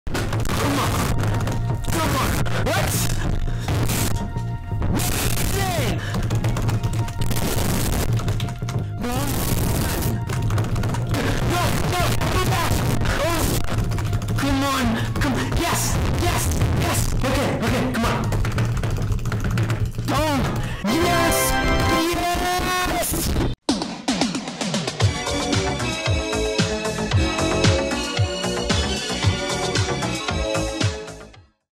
Rick Astley IMDb Speedrun in 0:20.40 [WR]
This is my Rick Astley IMDb page any% speedrun world record run. I hope you enjoy.